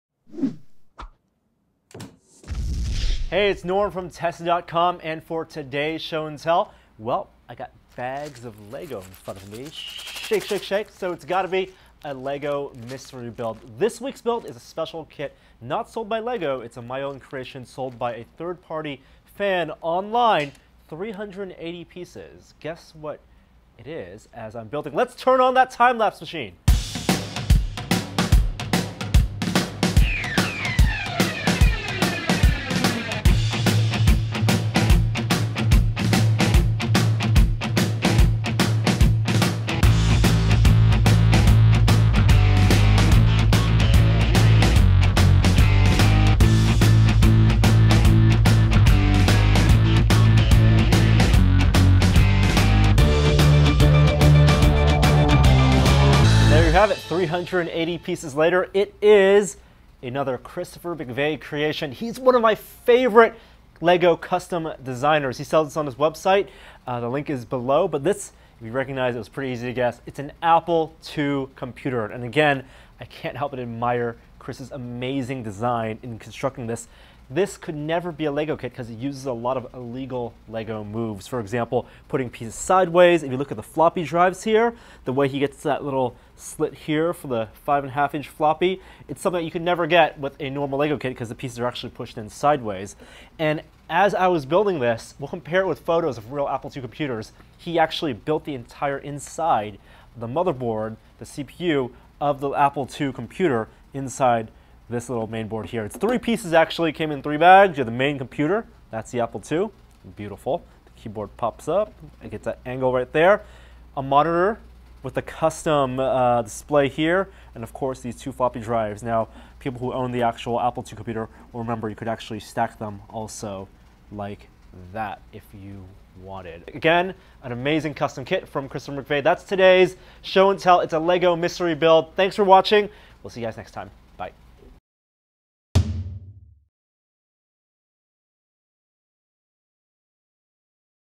Show and Tell: LEGO Mystery Build #7
Time for another mystery LEGO build! This week's kit comes from one of Norm's favorite custom LEGO designers, and is a wonderful tribute to an important piece of computer history. Place your best guess as to what's being built in the comments!